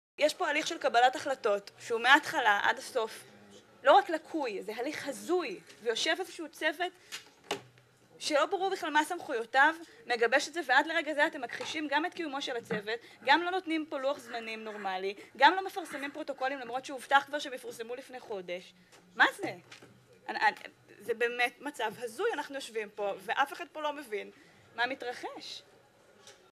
ערוץ הכנסת - "הליך הזוי" ח"כ שפיר על המו"מ שהתנהל עם חברות הגז ללא פרוטוקולים, 30.6.15
בוועדה לביקורת המדינה התגלה כי הצוות שניהל את המו"מ עם חברות הגז בדרך למתווה, התנהל ללא פרוטוקולים. ח"כ שפיר: "תהליך לקוי והזוי"